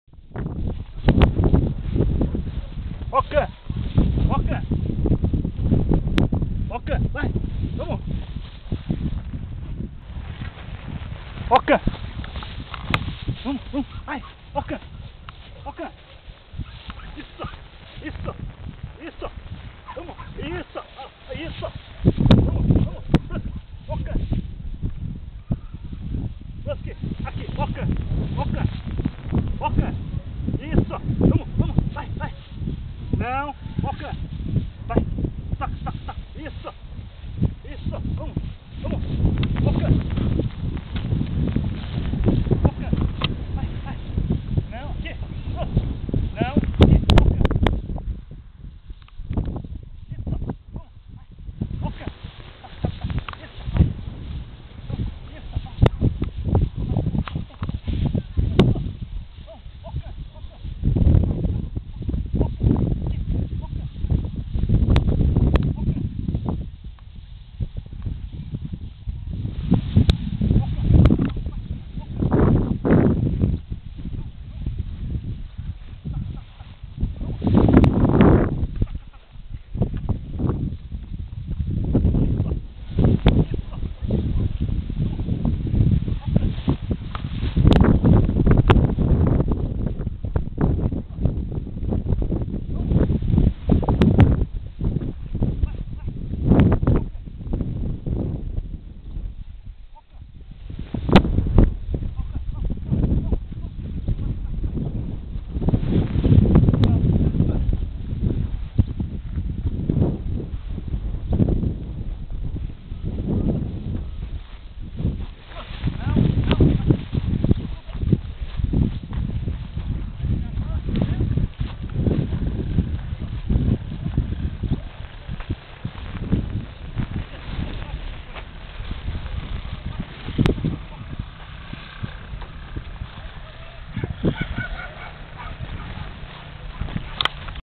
Border Collie Brusky
Nosso Border Collie Brusky trabalhando pastoreando o gado.